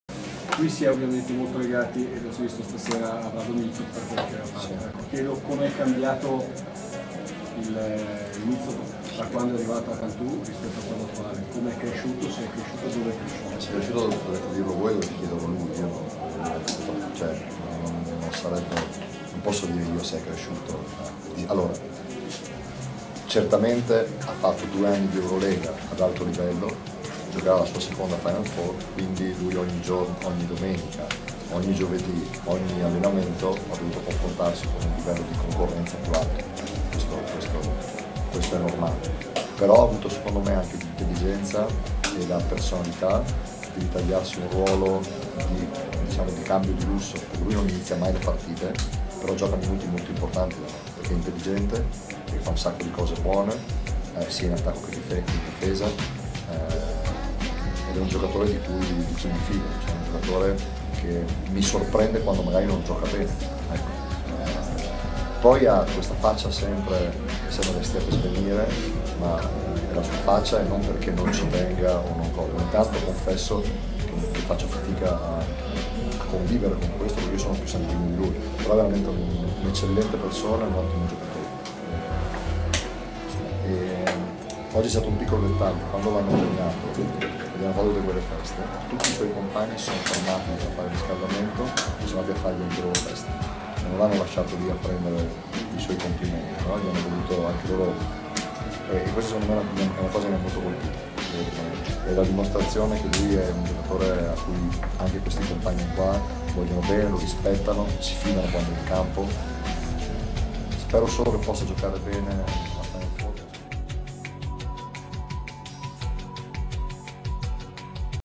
Vlado is back
ritorno al pianella per Vladimir Micov  per l'amichevole
Acqua Vitasnella Cantù - Cska Moskow
coach Ettore Messina  parla di Vlado